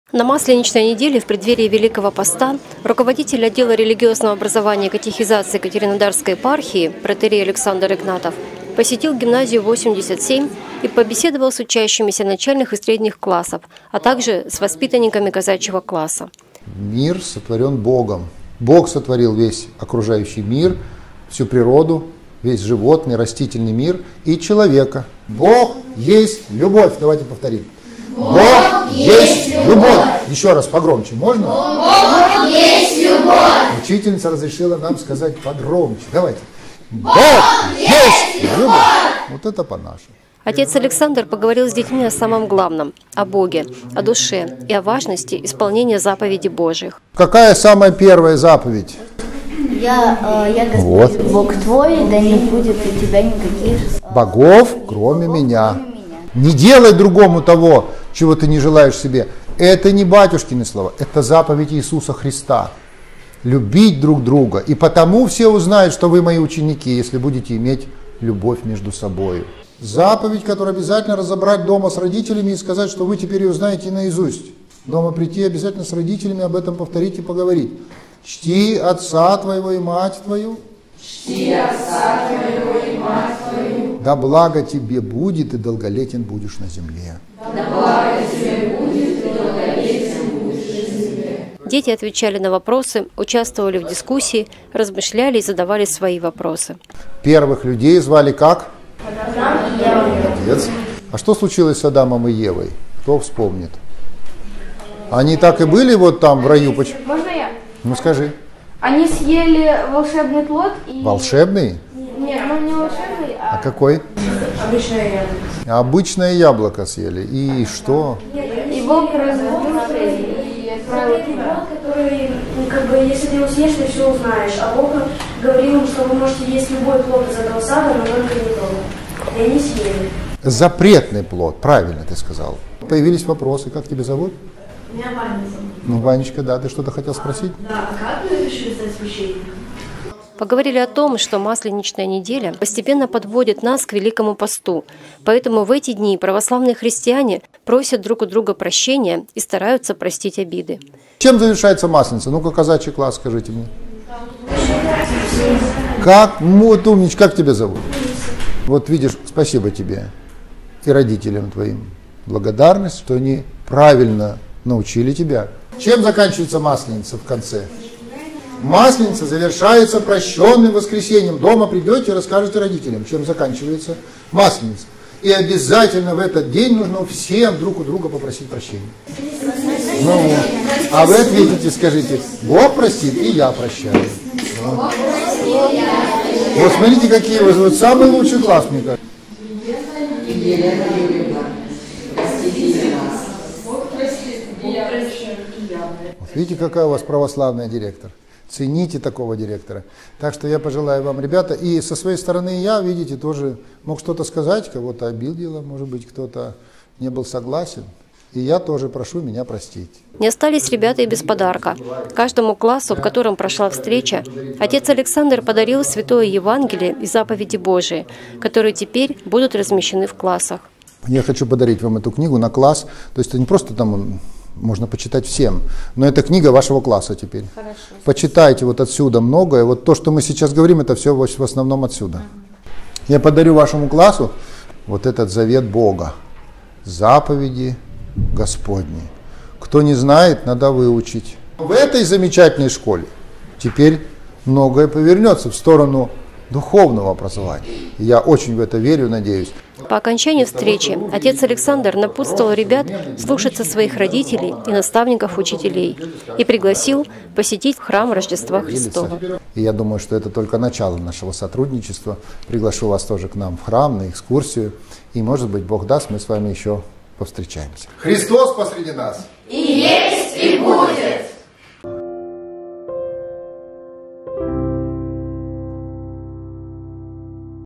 Встреча с учащимися Гимназии 87.
Руководитель ОРОиК протоиерей Александр Игнатов посетил и провел встречу с учащимися разных классов Гимназии 87.